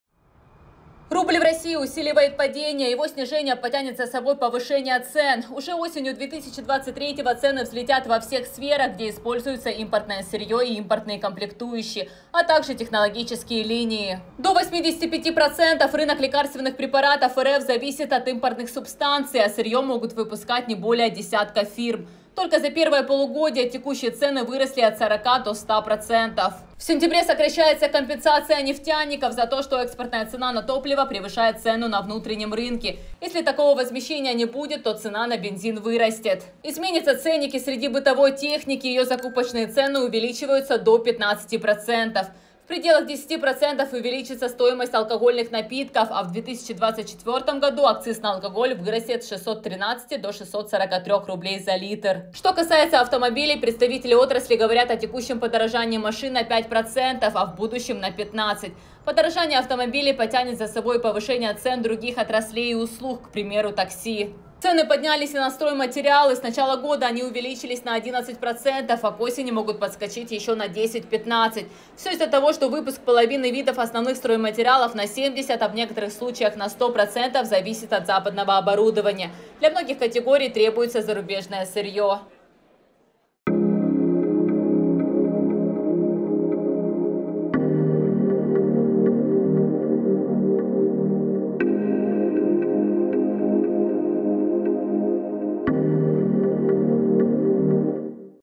В РФ пришла бедность. Рубль катится в пропасть, цены летят в космос
UA — До Росії прийшла економічна біда. На тлі найсильнішого падіння рубля ціни значно зростають. Коли росіяни відчують збільшення рівня бідності, дивіться у сюжеті.

RU — В Россию пришла экономическая беда. На фоне сильнейшего падения рубля цены значительно растут. Когда россияне ощутят увеличение уровня бедности, смотрите в сюжете.